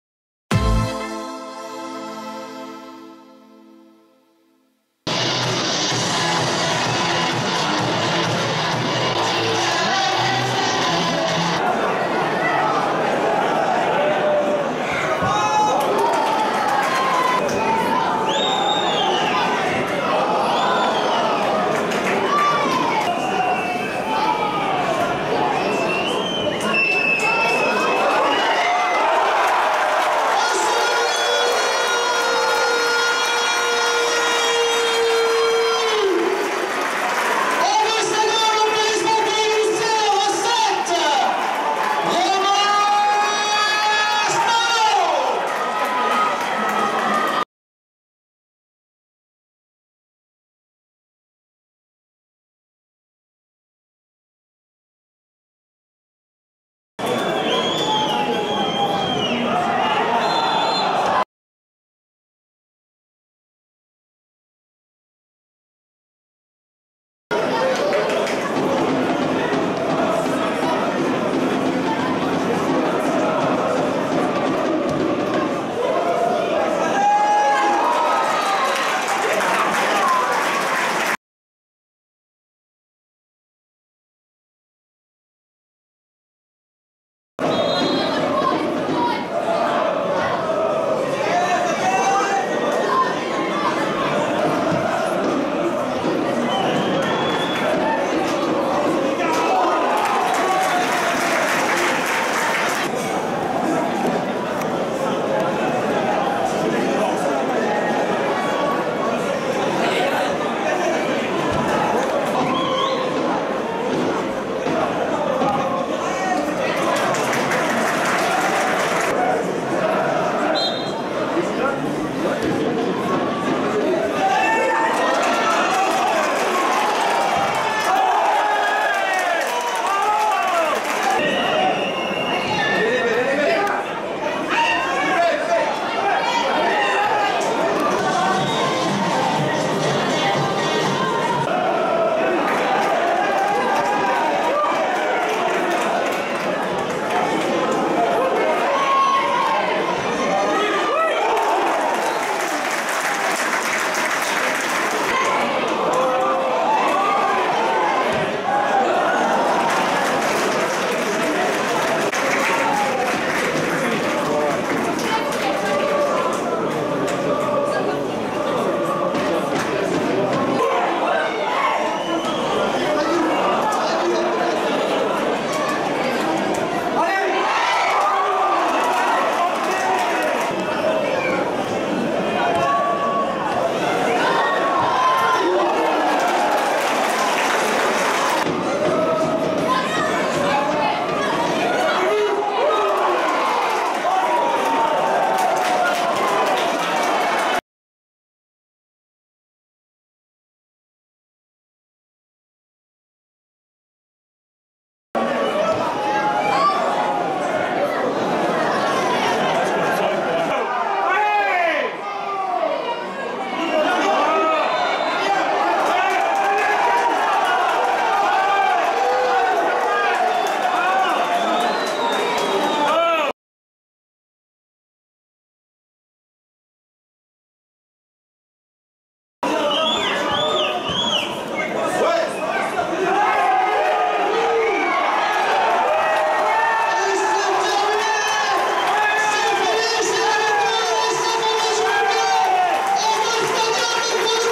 11' journée CFA ASF Andrézieux-Bouthéon 1-0 Grenoble F38 : le résumé
Revivez les temps forts de la 11ème journée de CFA entre l'ASF Andrézieux-Bouthéon et le GF38, victoire 1-0. But de Spano pour l'ASF.